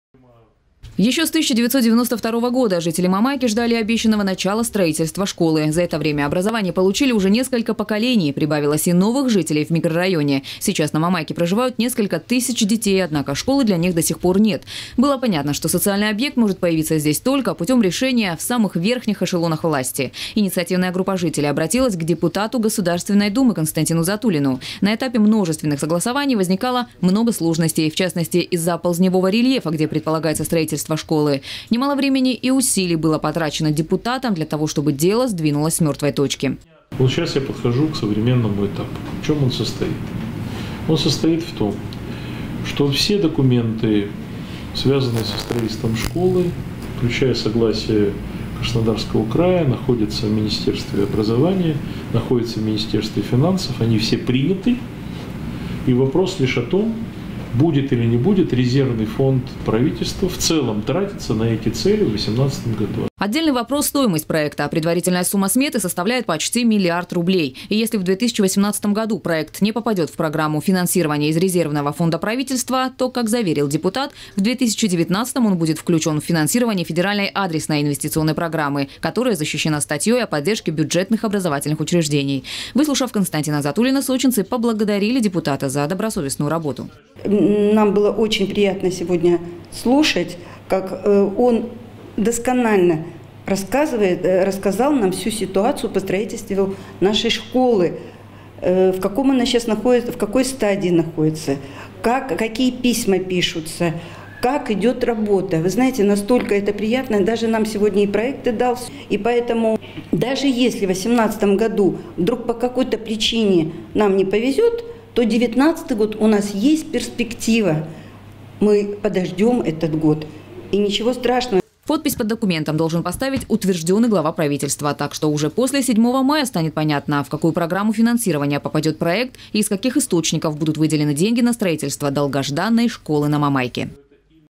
Депутат Госдумы Константин Затулин провёл в Сочи очередной прием граждан. Новости Эфкате
На этот раз встреча была специальна организована для жителей микрорайона Мамайка На повестке один вопрос — строительство школы на 800 мест на улице Ландышевой.

Ещё 1992 года жители Мамайки ждали обещанного начала строительства школы. За это время образование получили уже несколько поколений, прибавилось и новых жителей микрорайоне. Сейчас на Мамайке проживают несколько тысяч детей, однако школы для них до сих пор нет. Было понятно, что социальный объект может появиться здесь только путём решения в самых верхних эшелонах власти. Инициативная группа жителей обратилась к депутату Госдумы Константину Затулину. На этапе множественных согласований возникали сложности, в частности из-за оползневого рельефа, где предполагается строительство школы. Немало времени и усилий было потрачено депутатом для того, чтобы дело сдвинулось с мёртвой точки.